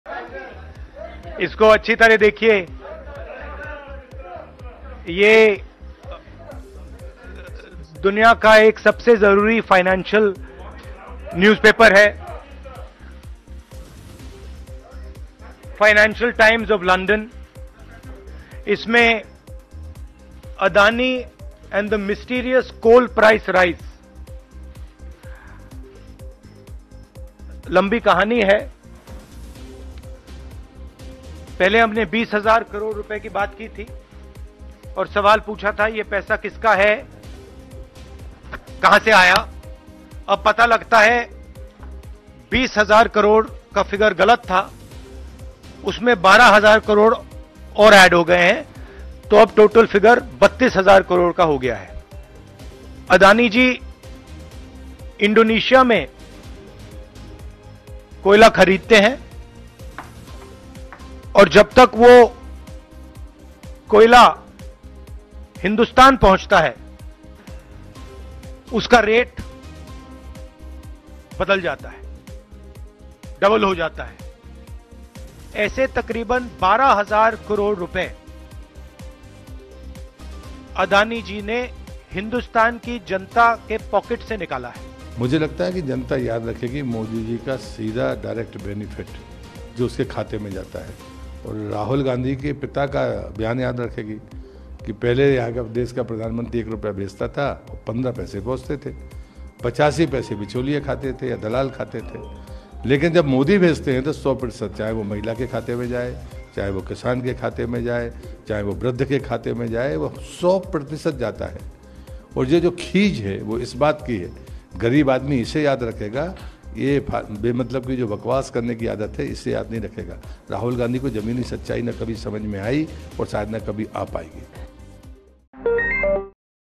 Rahul Gandhi के बयान पर केंद्रीय मंत्री प्रह्लाद सिंह पटेल का पलटवार
कांग्रेस सांसद राहुल गांधी आज प्रेस कॉन्फ्रेंस में गौतम अडानी पर हमला किया. राहुल ने लंदन के एक अखबार की रिपोर्ट का हवाला देते हए कहा कि अडानी ने 32 हजार करोड़ का घोटाला किया है. फाइनेंशियल टाइम्स की खबर दिखाकर राहुल ने आरोप लगाया कि ऐसे व्यक्ति को प्रधानमंत्री मोदी का संरक्षण प्राप्त है.